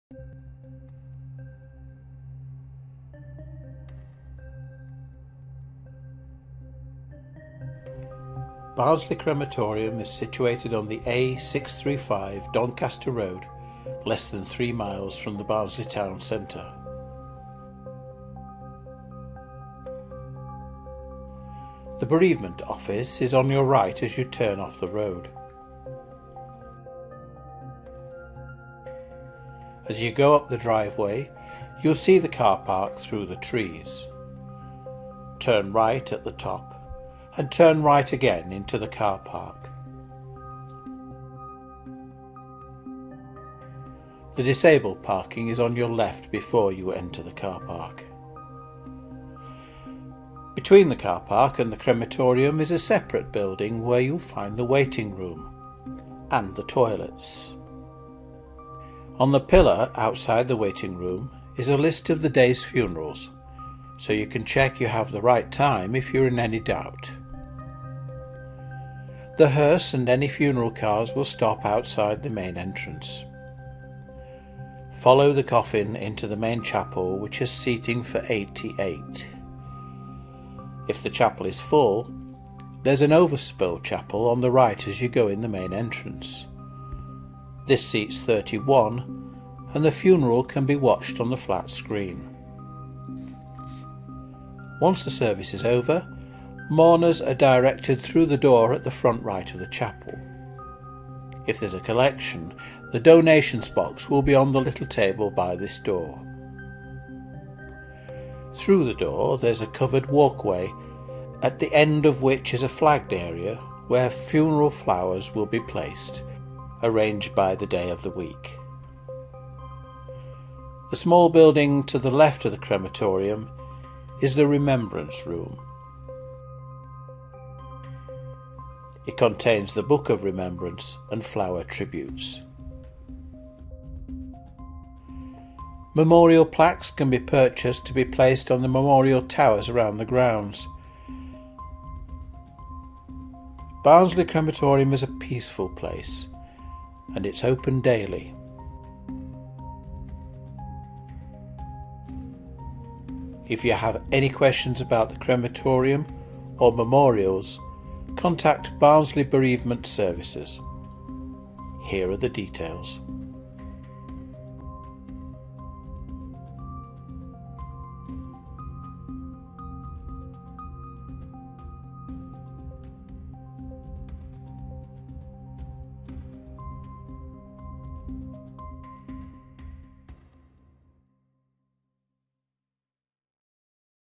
Barnsley Crematorium
A quick video guide to the crematorium. This should be useful for anyone who has not visited it before but who needs to attend a funeral there.